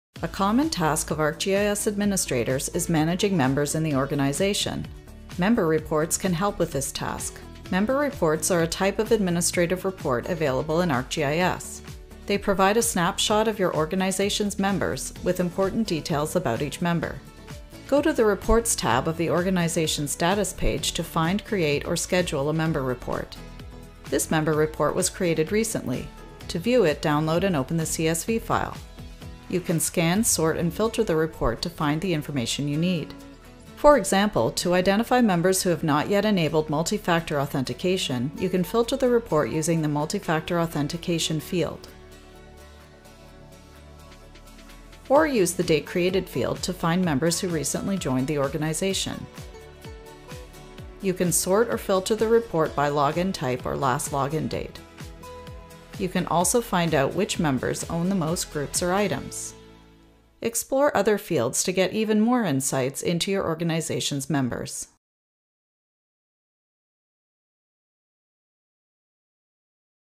What's in a Member Report?
A common task of ArcGIS administrators is managing members in the organization. The member report is a useful tool that helps with this task. In this GIS in a Minute video, you’ll learn what member reports are, how to find and use the information they contain, and how to access them. Note: This GIS in a Minute video features an ArcGIS Online organization, but the information covered also applies to ArcGIS Enterprise. Some of the user interface shown in the video, such as the status dashboard, looks different in an ArcGIS Enterprise organization.

Engage with Esri on Social Media!